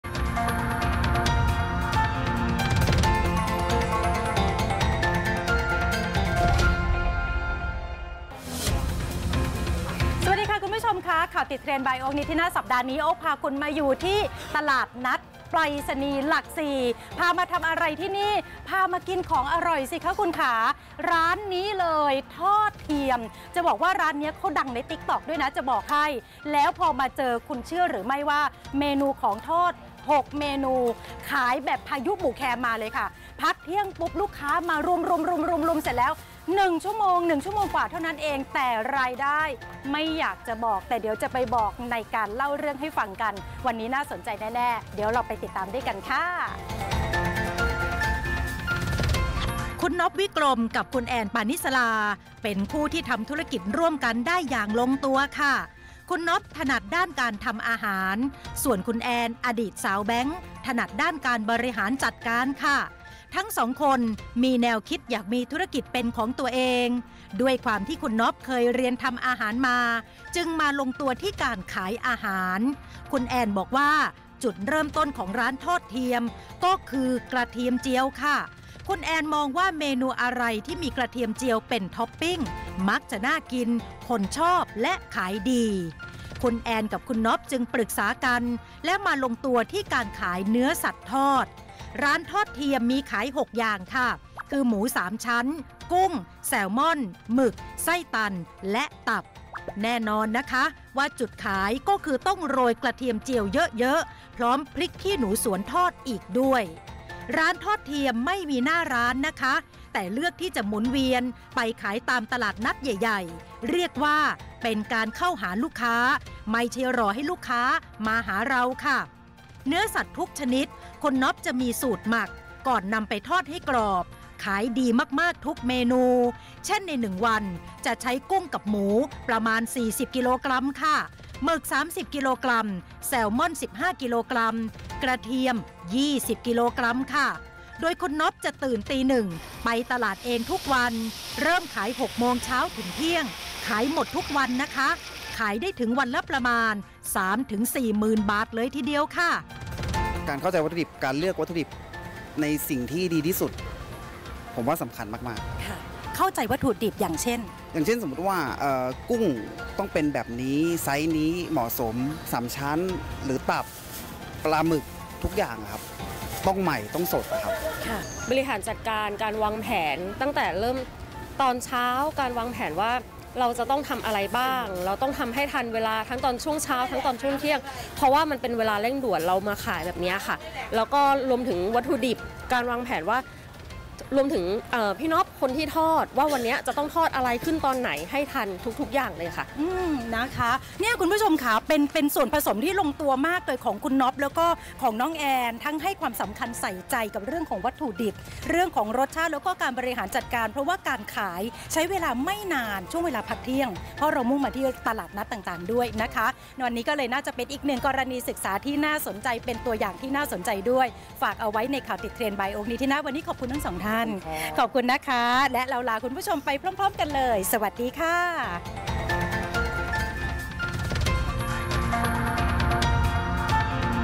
ข่าวติดเทรนด์ by โอ๊ค นิธินาฏ : ร้านทอดเทียม จุดเริ่มต้นจากกระเทียมเจียว
ข่าวติดเทรนด์ by โอ๊ค นิธินาฏ... คุณน็อบ วิกรม กับ คุณแอน ปาณิสรา เป็นคู่ที่ทำธุรกิจร่วมกันได้อย่างลงตัว คุณน็อบ ถนัดด้านการทำอาหาร ส่วนคุณแอน อดีตสาวแบงค์ ถนัดด้านการบริหารจัดการ ทั้งสองคนมีแนวคิดอยากมีธุรกิจเป็นของตัวเอง ด้วยความที่คุณน็อบเคยเรียนทำอาหารมา จึงมาลงตัวที่การขายอาหาร

คุณแอน บอกว่า จุดเริ่มต้นของ ร้านทอดเทียม ก็คือ กระเทียมเจียว คุณแอนมองว่า เมนูอะไรที่มีกระเทียมเจียวเป็นท็อปปิ้ง มักจะน่ากิน คนชอบและขายดี

คุณแอนกับคุณน็อบ จึงปรึกษากันและมาลงตัวที่การขายเนื้อสัตว์ทอด ร้านทอดเทียม มีขาย 6 อย่าง คือ หมูสามชั้น กุ้ง แซลมอน หมึก ไส้ตัน และตับ แน่นอนว่าจุดขายก็คือต้องโรยกระเทียมเจียวเยอะ ๆ พร้อมพริกขี้หนูสวนทอดด้วย

ร้านทอดเทียมไม่มีหน้าร้าน แต่เลือกที่จะหมุนเวียนไปขายตามตลาดนัดใหญ่ ๆ เรียกว่าเป็นการเข้าหาลูกค้า ไม่ใช่รอให้ลูกค้ามาหาเรา

เนื้อสัตว์ทุกชนิด คุณน็อบจะมีสูตรหมัก ก่อนนำไปทอดให้กรอบ ขายดีมาก ๆ ทุกเมนู เช่น ใน 1 วัน จะใช้กุ้งกับหมูประมาณ 40 กิโลกรัม, หมึก 3 กิโลกรัม, แซลมอน 15 กิโลกรัม, กระเทียม 20 กิโลกรัม โดยคุณน็อบจะตื่นตี 1 ไปตลาดเองทุกวัน เริ่มขาย 6 โมงเช้าถึงเที่ยง ขายหมดทุกวัน ขายได้ถึงวันละประมาณ 3-40,000 บาทเลยทีเดียว

รายการ ขันข่าวเช้าตรู่เสาร์ - อาทิตย์
ออกอากาศ ทุกวันเสาร์ - อาทิตย์ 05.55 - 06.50 น.